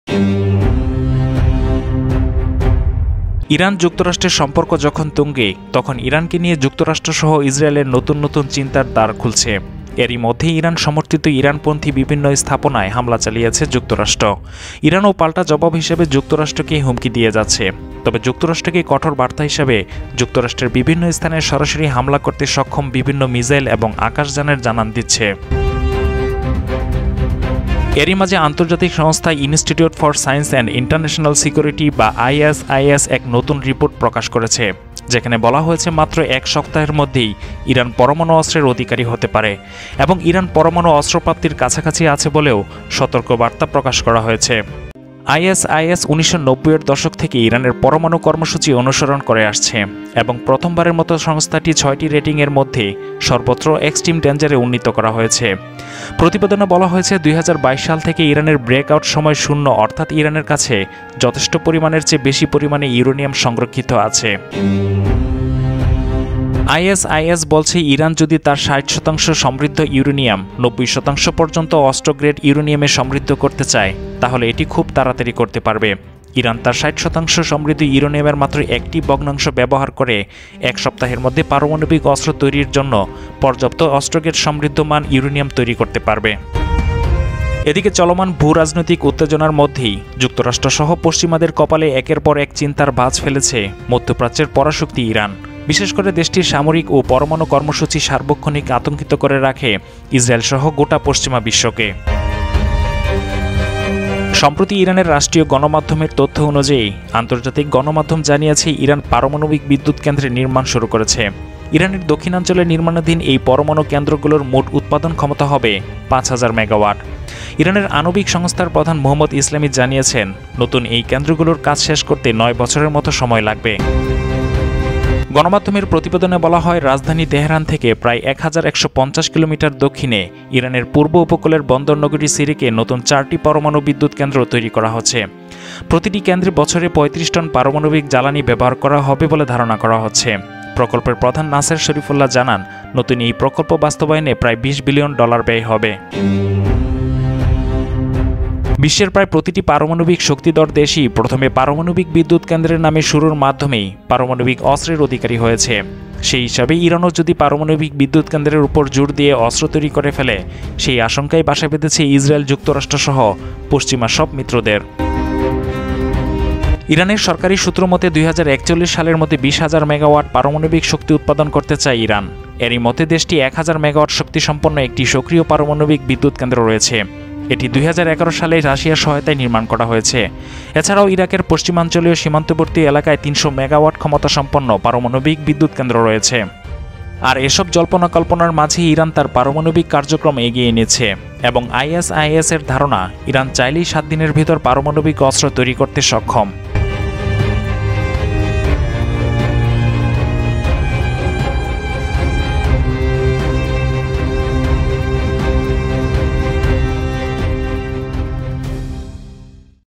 এবার মাত্র ৭ দিনেই পরমানু অস্ত্রের অধিকারী ইরান
ইরান যুক্তরাষ্ট্র সম্পর্ক যখন তুঙ্গে তখন ইরান কে নিয়ে যুক্তরাষ্ট্র সহ ইসরায়েলের নতুন নতুন চিন্তার দাড় খুলছে। এর ই মধ্যে ইরান সমর্থিত ইরান পন্থী বিভিন্ন স্থাপনায় হামলা চালিয়েছে যুক্তরাষ্ট্র। ইরান ও পাল্টা জবাব হিসেবে যুক্তরাষ্ট্র কে হুমকি দিয়ে ই যাচ্ছে। তবে যুক্তরাষ্ট্রকে কঠোর বার্তা হিসেবে যুক্তরাষ্ট্রের বিভিন্ন স্থানে সরাসরি হামলার করতে সক্ষম বিভিন্না মিসাইল এবং আকাশযানের জানান দিচ্ছে ।